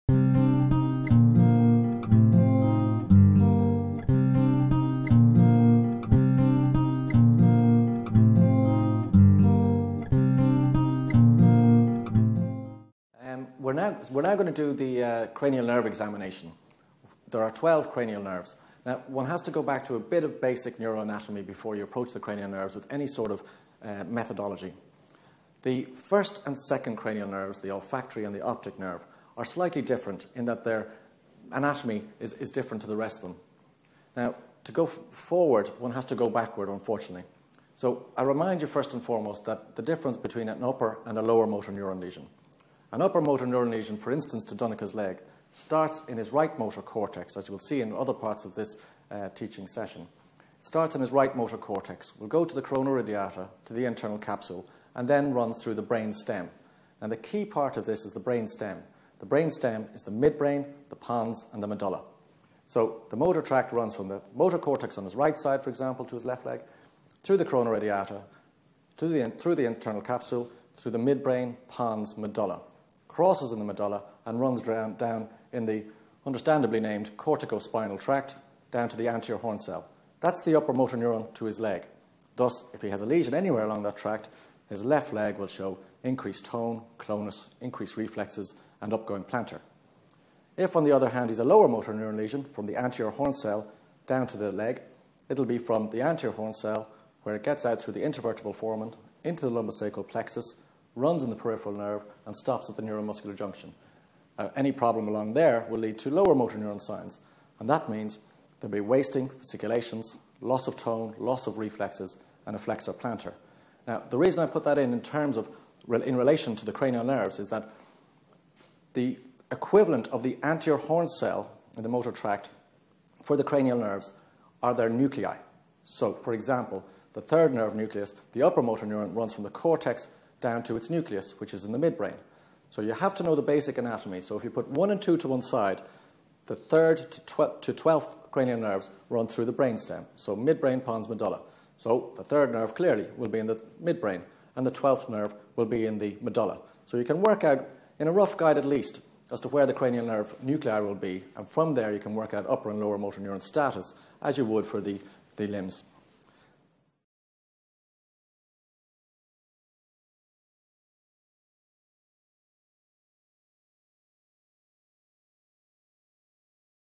Neurology - Topic 1 - Brief neuro anatomical background
Brief neuro anatomical background given by Professor Tubridy